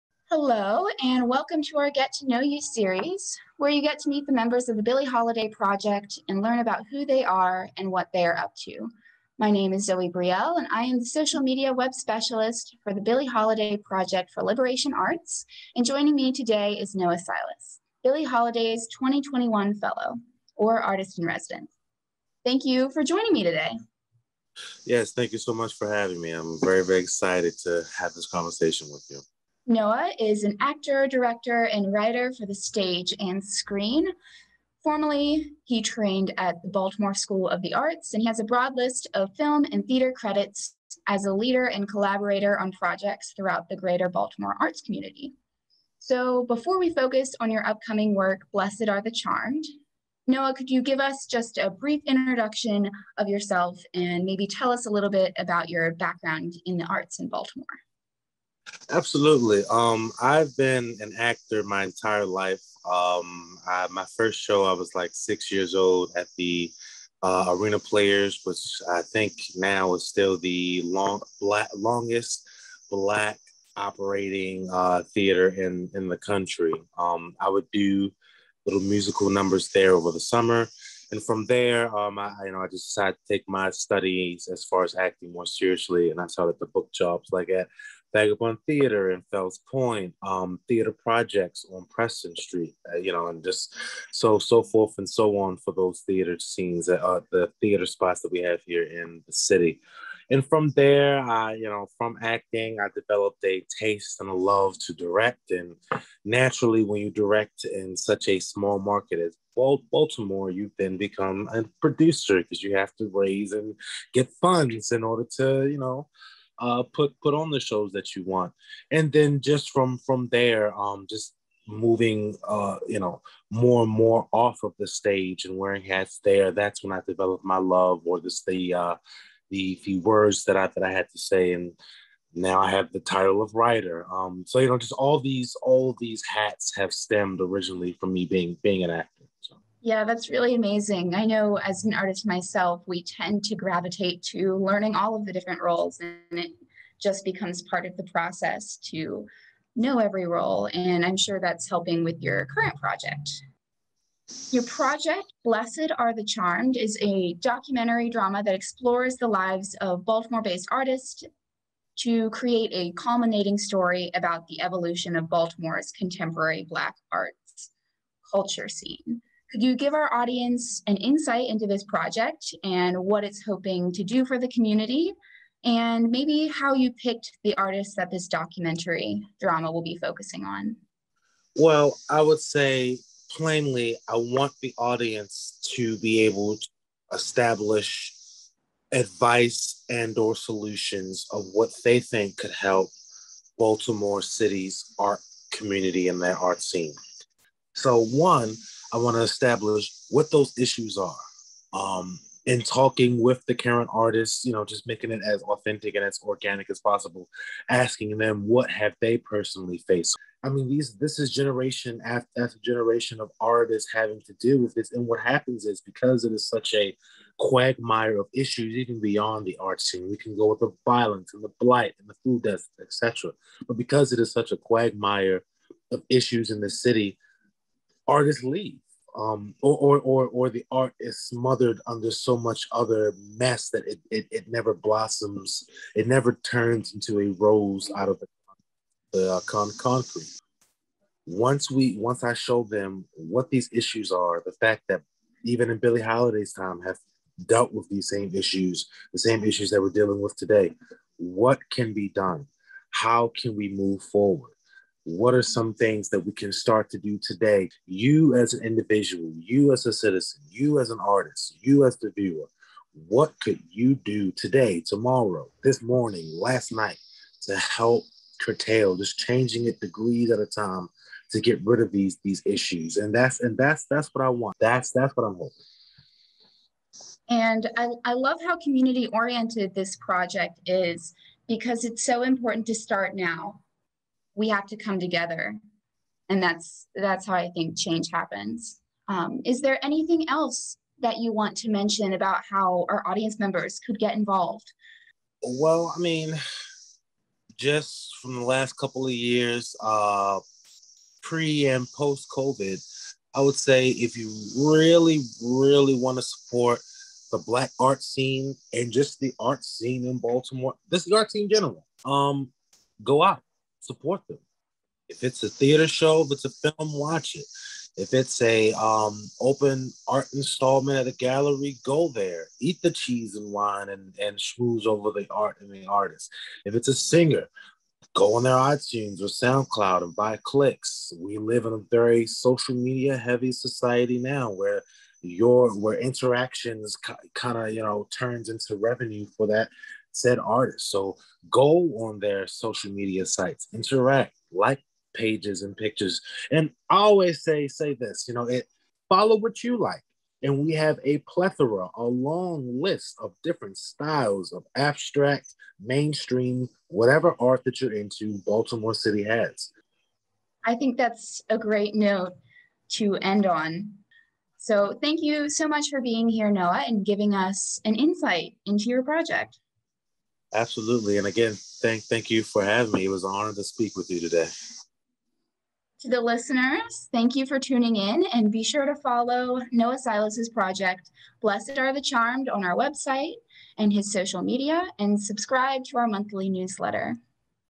Noah Silas- Get to Know the Members of the Billie Holiday Project for Liberation Arts
Billie Holidays Podcast Series
A conversation with Noah Silas moderated by Zoë Brielle Payne.